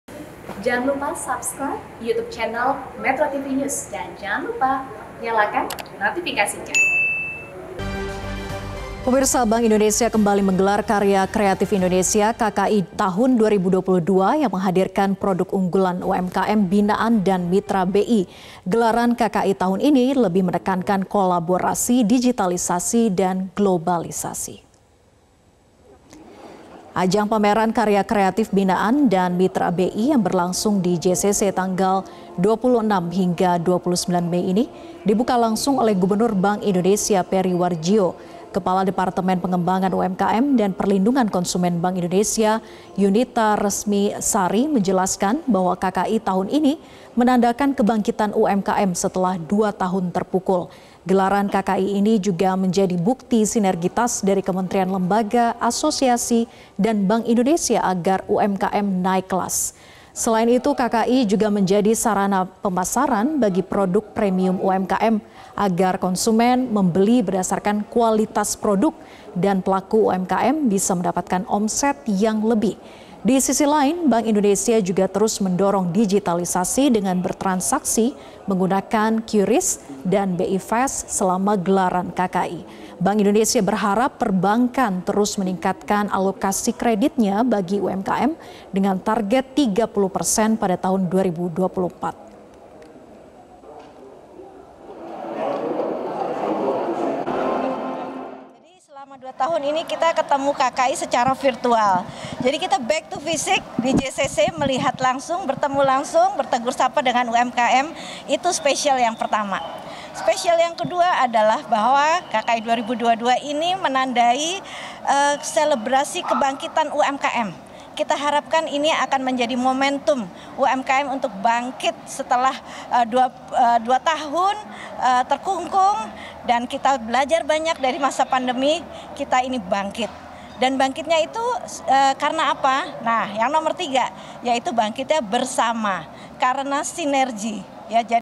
Bank Indonesia Gelar Karya Kreatif Indonesia 2022 di JCC Jakarta
Bank Indonesia kembali menggelar Karya Kreatif Indonesia 2022, yang menghadirkan produk unggulan UMKM binaan dan mitra BI. Gelaran KKI tahun ini, lebih menekankan kolaborasi digitalisasi dan globalisasi. Ajang pameran kreatif yang diselenggarakan di JCC Jakarta 26-29 Mei 2022, dibuka langsung oleh Gubernur Bank Indonesia, Perry Warjiyo.

Kepala Departemen Perkembangan UMKM dan Perlindungan Konsumen Bank Indonesia, Yunita Resmi Sari menjelaskan, bahwa KKI tahun ini menandakan kebangkitan UMKM setelah dua tahun terpukul. Gelaran KKI ini juga menjadi bukti sinergitas dari Kementerian, Lembaga Asosiasi dan Bank Indonesia agar UMKM naik kelas.